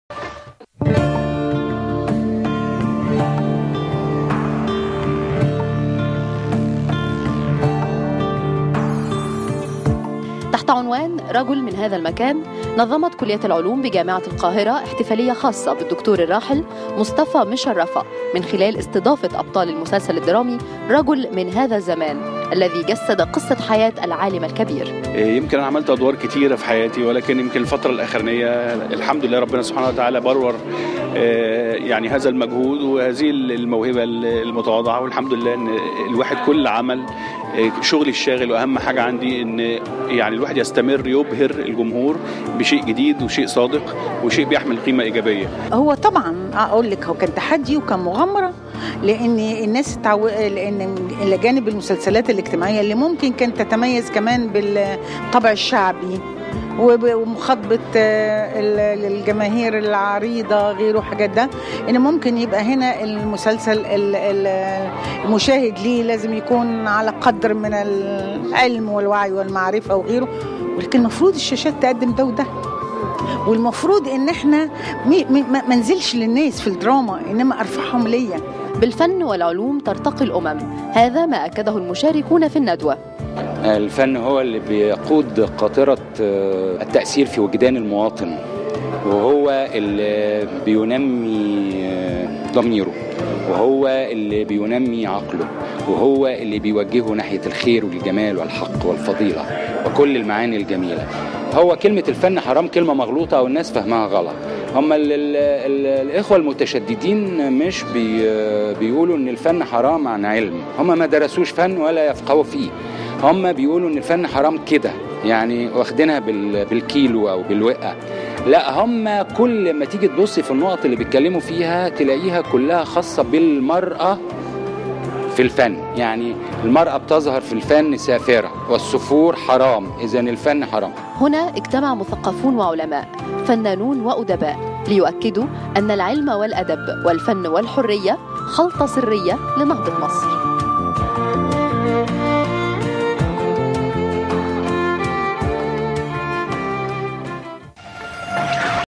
إحتفالية بالدكتور الراحل مصطفى مشرفة
نظمت كلية العلوم جامعة القاهرة إحتفالية خاصة بالدكتور الراحل مصطفى مشرفة